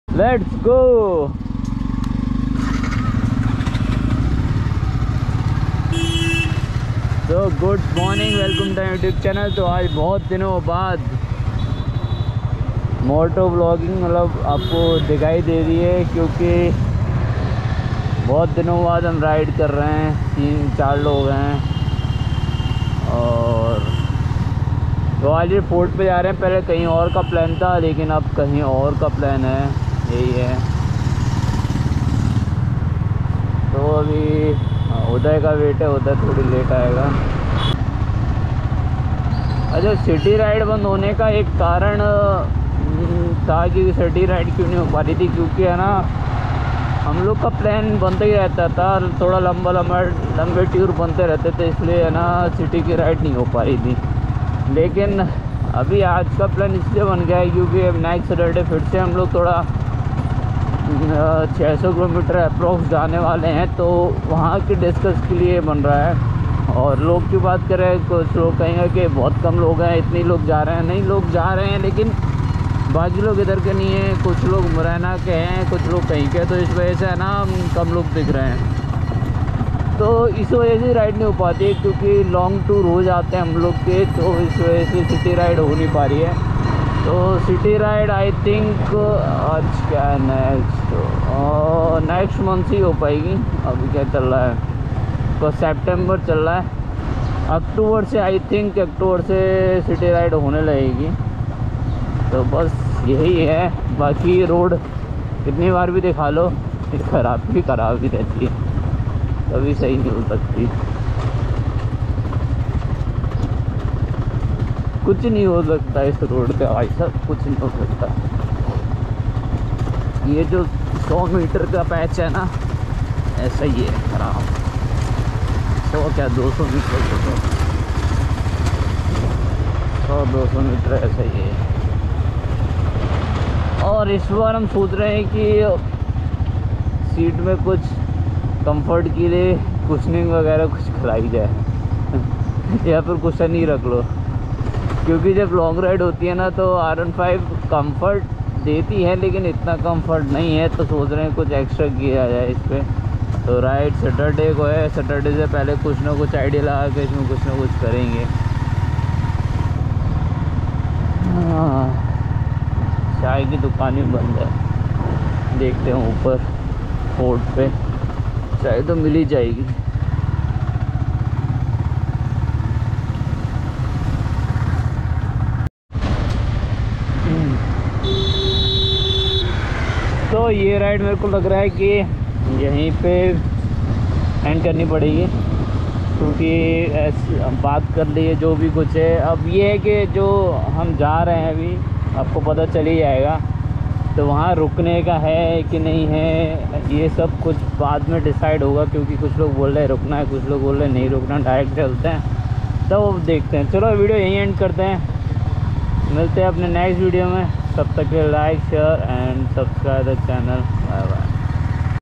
Plan to new ride ✌️🏍️
to city ride ki video nahi ban la rahi thi kyunki long ride ho rahi thi to ye video main bhi maine long ride ke bare main bataya h and jald hi next ride ke baad fir city ride ke video banaya karunga kyunki city ride ke video kam ho gaye the thanks for watching like share and subscribe.